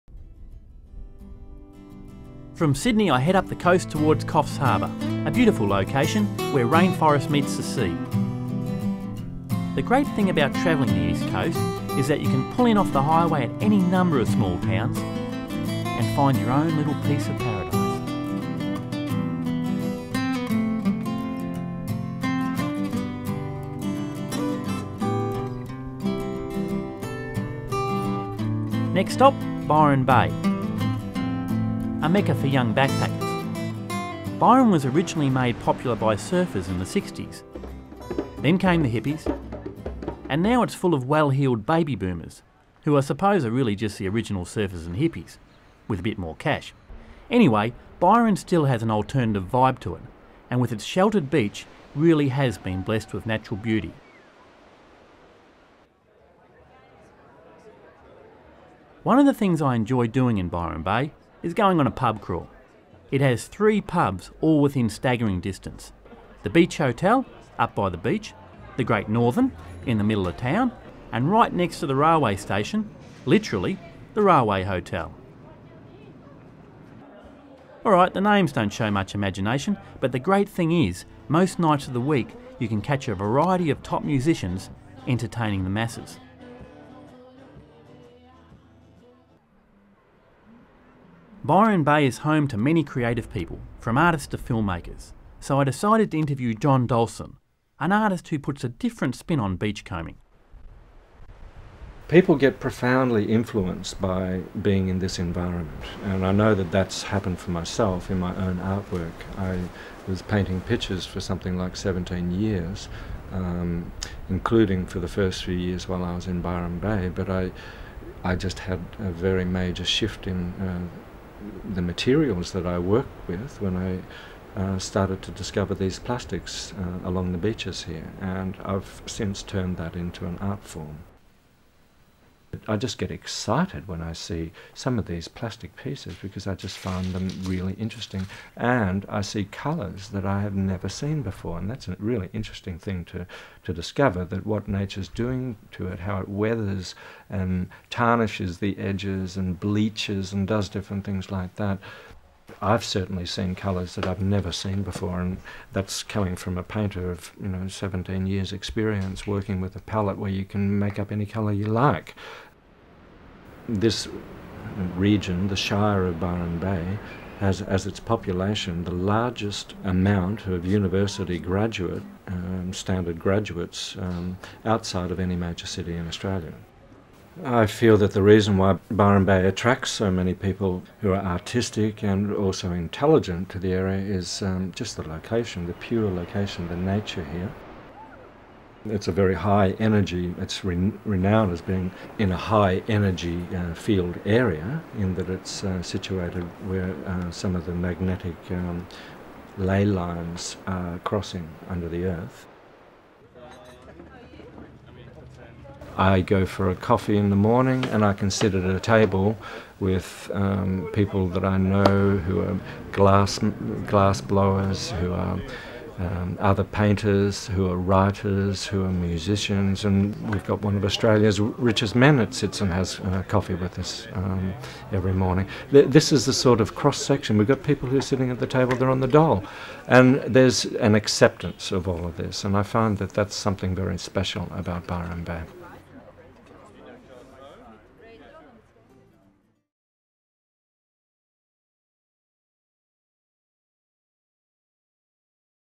Byron Bay, Beach Art - Australia Travel HD
It seems every backpacker must make the pilgrimage to Byron Bay. Once the centre for peace, love and alternative lifestyles, now home of a growing number of baby boomer 'seachangers' from capital cities down south. With this new mix, Byron is evolving, (and the real estate prices are going through the roof).

Byron attracts creative types and also has a huge collection of practitioners of alternative health therapies. To try and capture this I am interviewing John Dahlsen an artist. John makes a daily trip to the beach where he collects plastic waste washed up the night before. From this waste he creates art pieces. I found his art to be like a double edged sword, I was both astounded by the beauty of his art, and repelled by the thought it was all collected from our beaches.